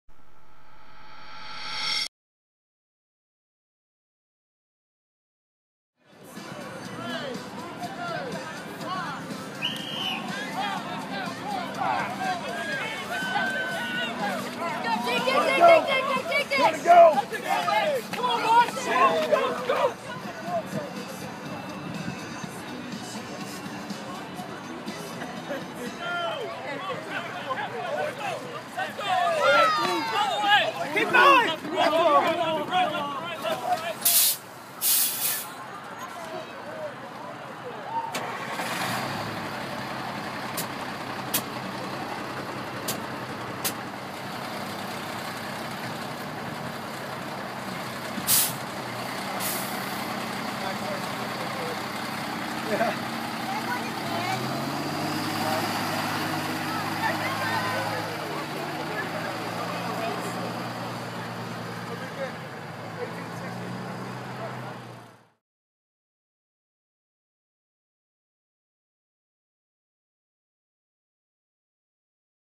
Pink Lady Fire Truck Pull 2014
3rd Annual Pink Lady Fire Truck Pull at the N.C. Music Factory. Charlotte-Mecklenburg Police Department's "Cops 4 the Cure", joined by the Charlotte Fire Department's "Fight to Extinguish" organization, support those that are battling cancer and other life threatening diseases.